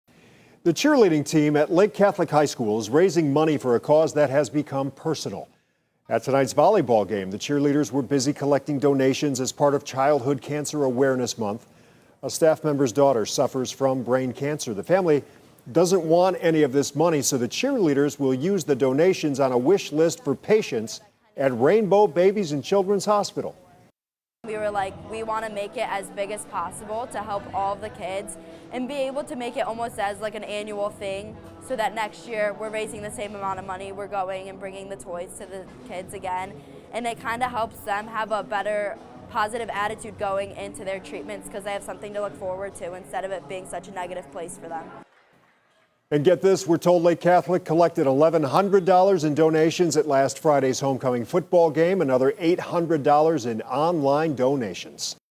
Lake Catholic raises money for childhood cancer
The cheerleading team at Lake Catholic High School is raising money for a cause that has become personal.